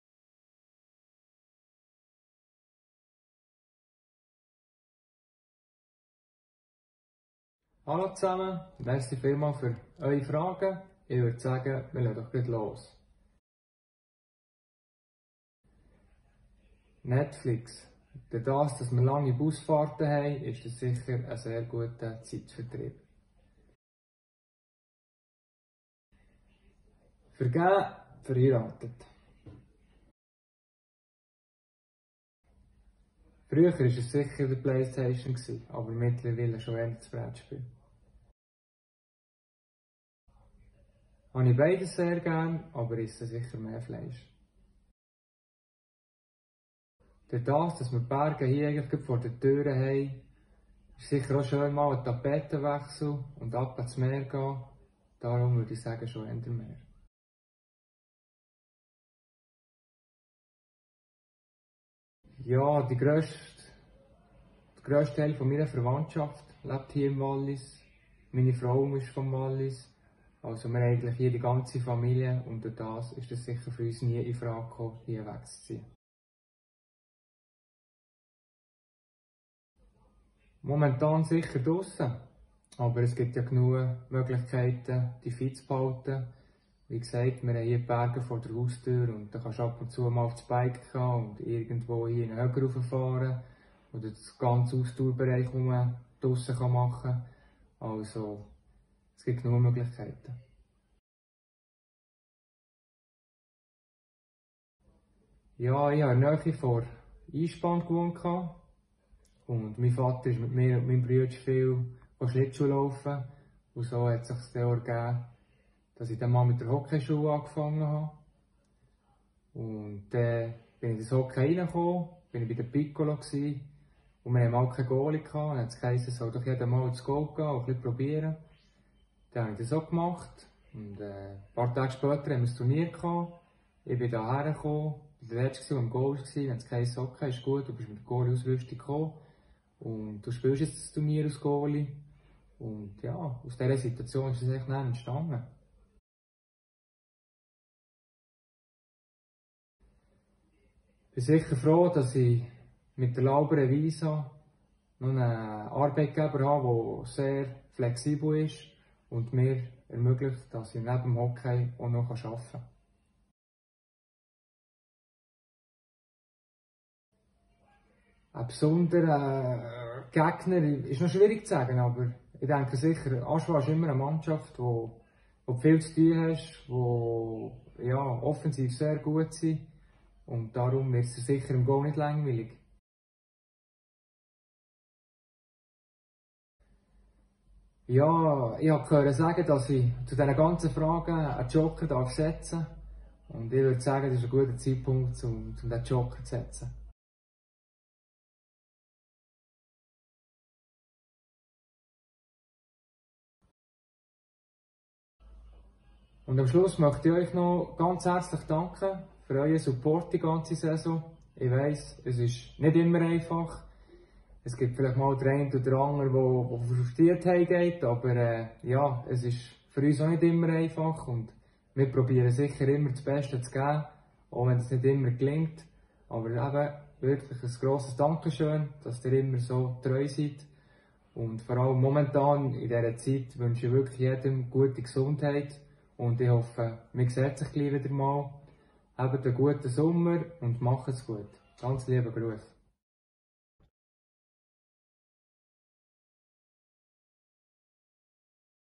10 Fragen an... Reto Lory
Spieler des EHC Visp beantworten Fragen der Fans.